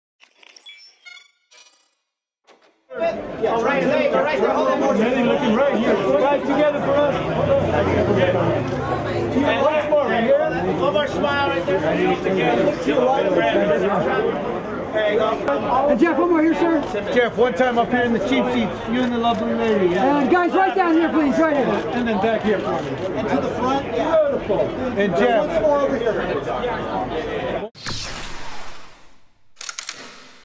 98 Degrees Jeff Timmons & Amanda Timmons SyFy "Dead 7" Movie Premiere
BROLL: 98 Degrees Jeff Timmons and Amanda Timmons arrive on the red carpet at the SyFy original movie "Dead 7" premiere held at Harmony Gold Theater in Los Angeles, Ca April 1, 2016